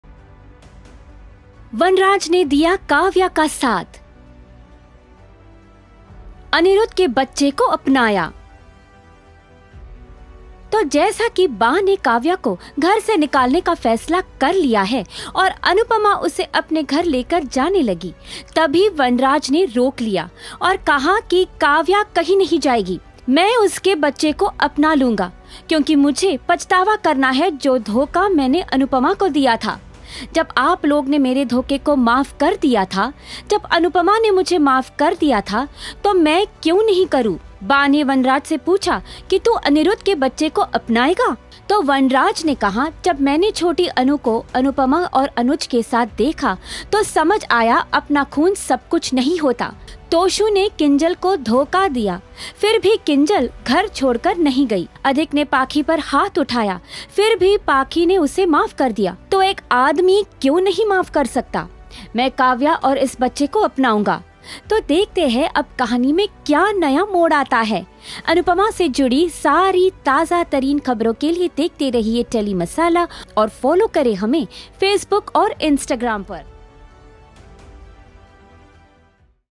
Vanraj Go Against Baa, Accepts Kavya & Her Illegitimate Baby | Episode Update
Vanraj is finally ready to accept Kavya's illegitimate baby, He goes against Baa and make everyone understand why is he supporting her